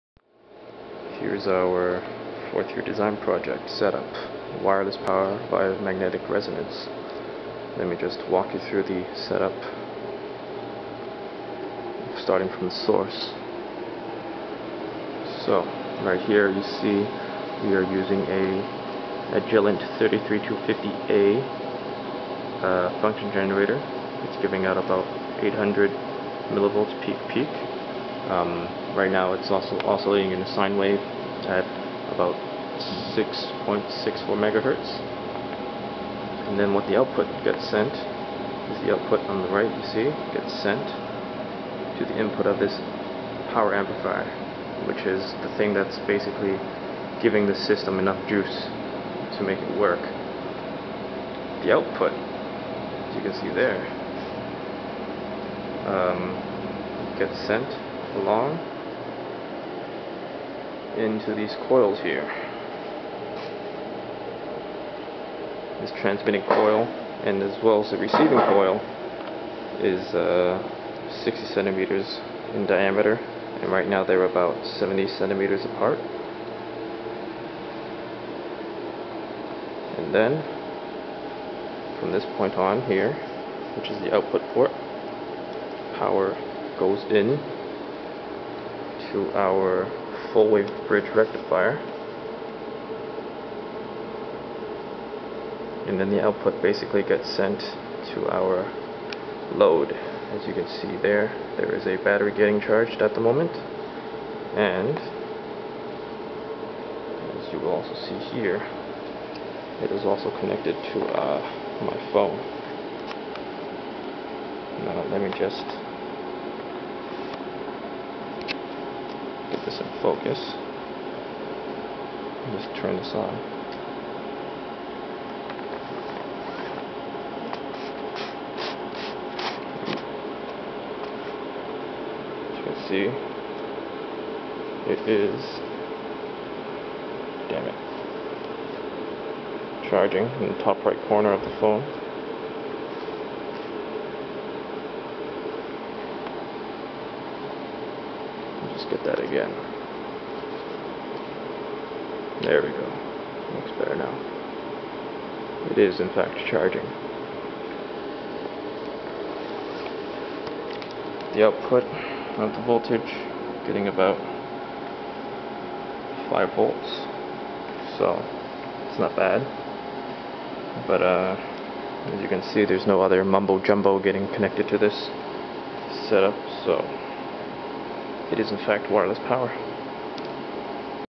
Wireless Power Transfer via Magnetic Resonance to Charge Cell Phone.AVI
This is a video of our 4th year design project. We're successfully charged a cell phone battery and cell phone wirelessly over at distance of about 70cm. Unfortunately, anything beyound this we won't be able to charge. lol